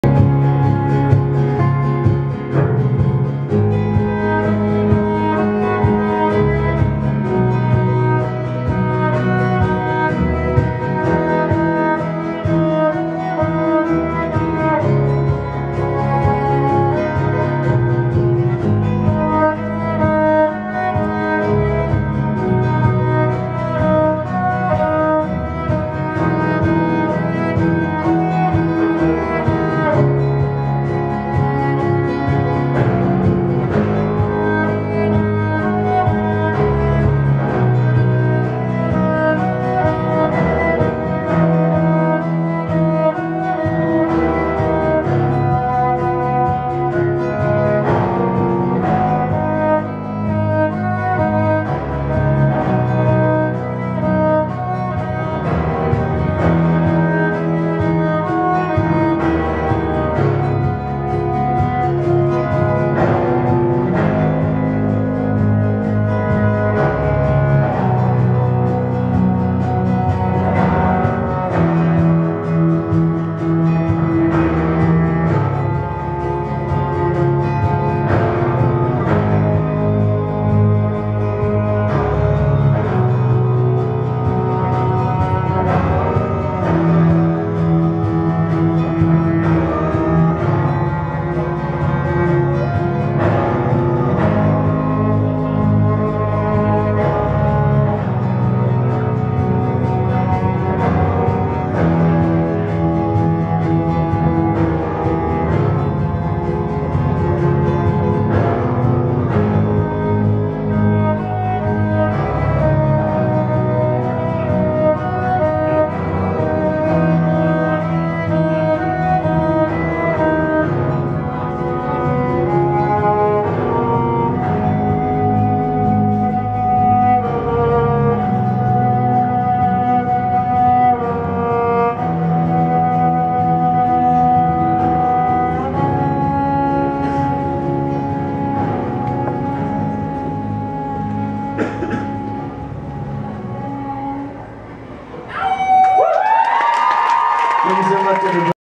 Greg Laswell and Austin Hartley-Leonard at the Crocodile
Greg Laswell (guitar, vox) and Austin Hartley-Leonard (cello) at the Crocodile in Seattle, during the recent west coast tour with Lenka in April 2011